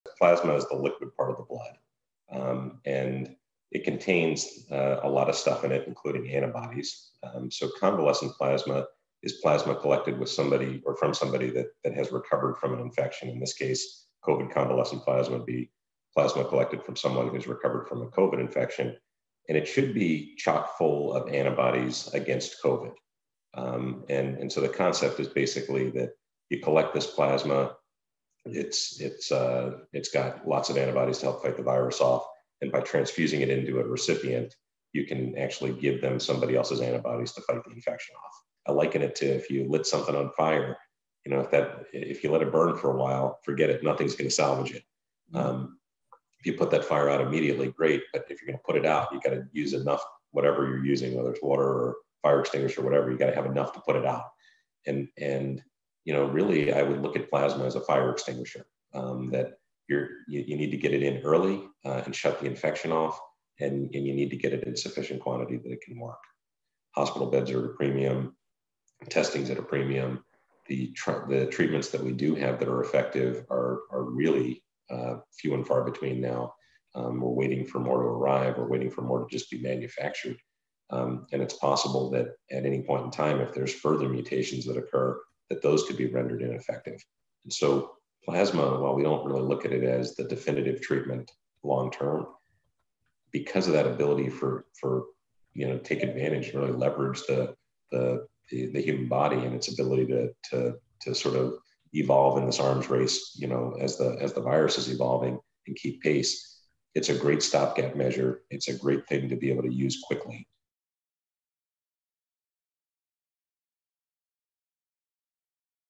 Convalescent plasma’s potential as COVID-19 treatment
Using plasma of patients convalesced from COVID-19, whose blood contains antibodies against the disease-causing SARS-CoV-2 virus, has been touted as a promising treatment for some patients with signs of infection.